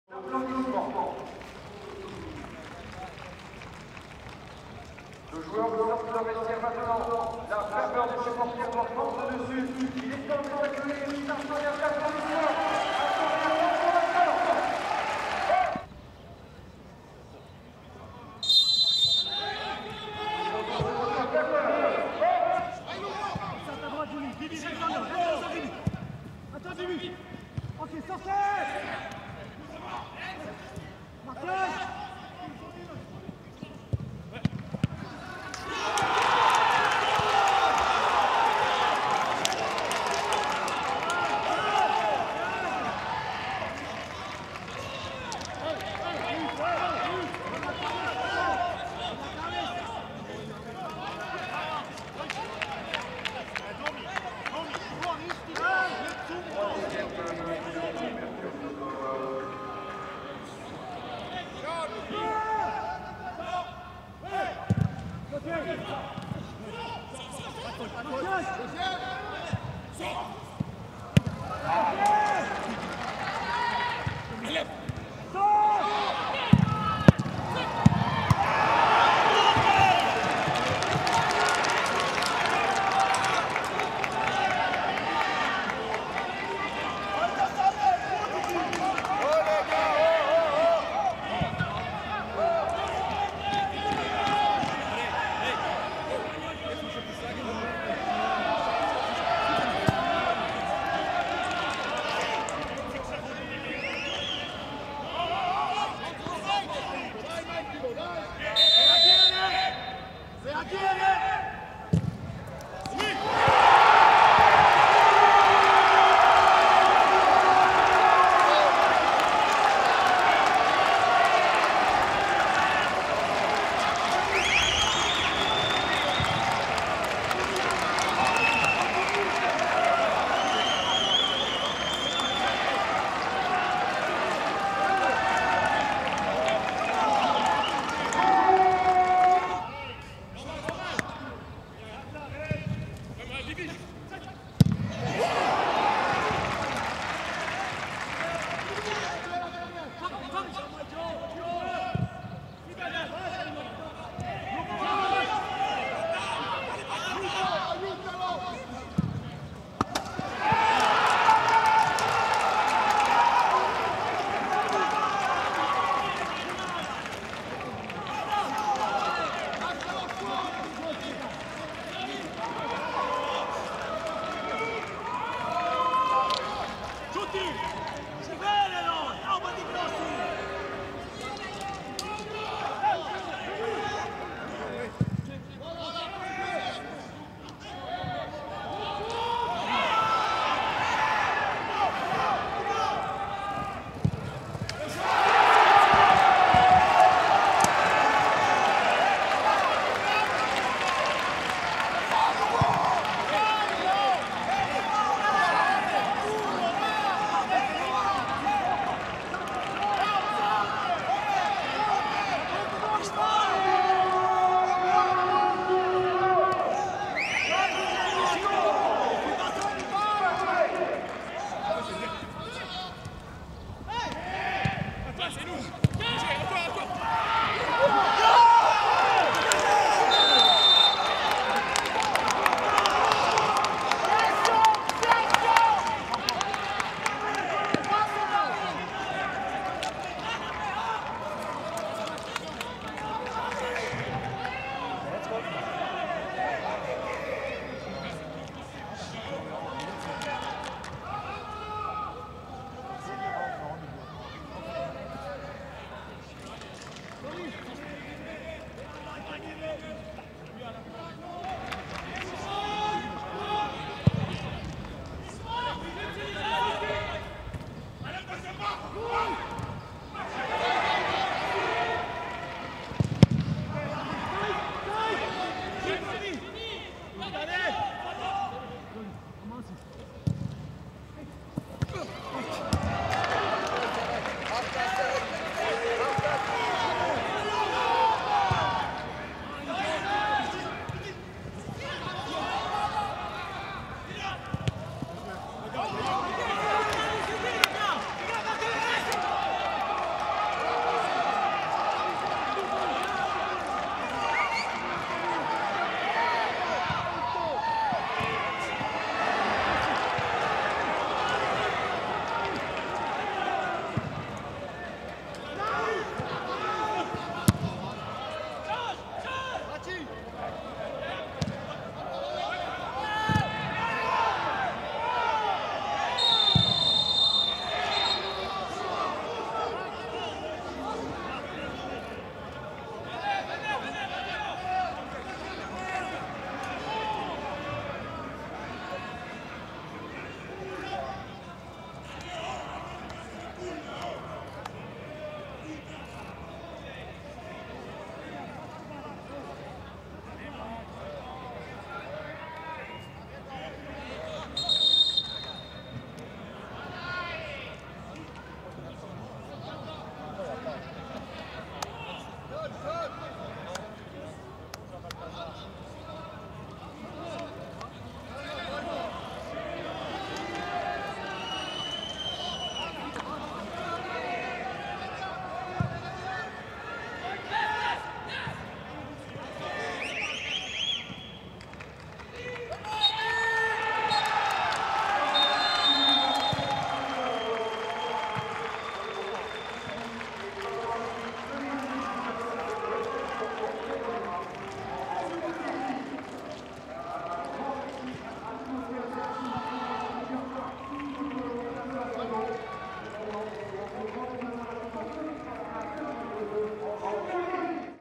FC Sion - FC Thoune (2-3) | Les Highlights
Revivez les meilleurs moments du dernier match de la saison 2020/2021. Malgré la défaite, les Valaisans évolueront encore en Super League !